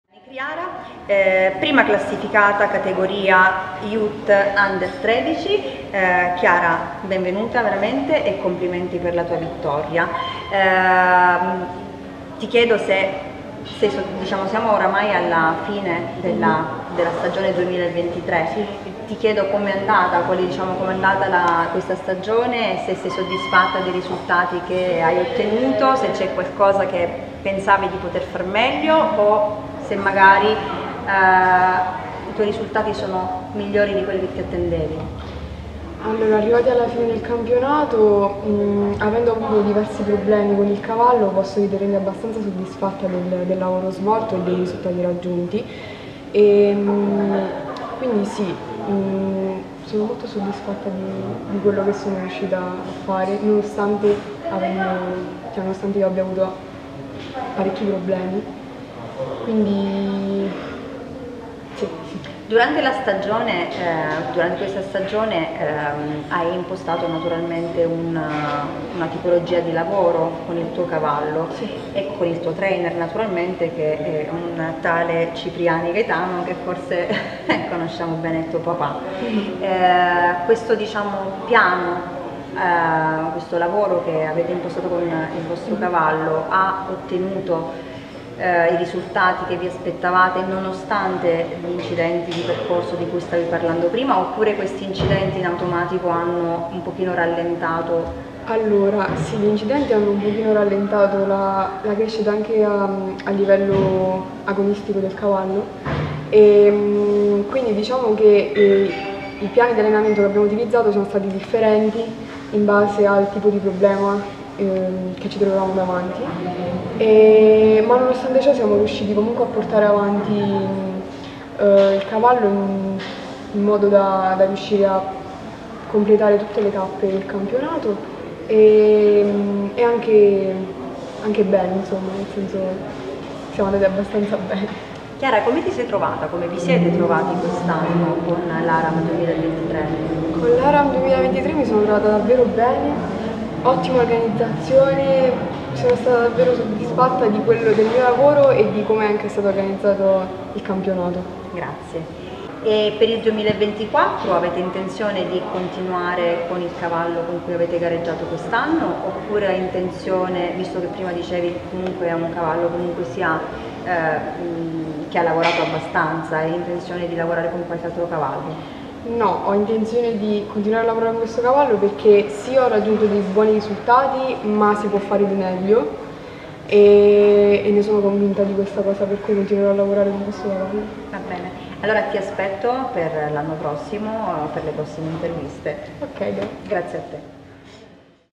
Interviste 4 show ARAM 2023 - CHIARA CIPRIANI
Champion Youth 13&Under - South Italy Futurity e 4 show ARAM-IRHA-FISE-NRHA 2023 - credits Bonaga Communication

YOUTH 13&UNDER:
1) CIPRIANI CHIARA & AC CUSTOM HICORY owner CIPRIANI GAETANO score 68,5

GREEN REINER 2:
4) CIPRIANI CHIARA & AC CUSTOM HICORY owner CIPRIANI GAETANO score 65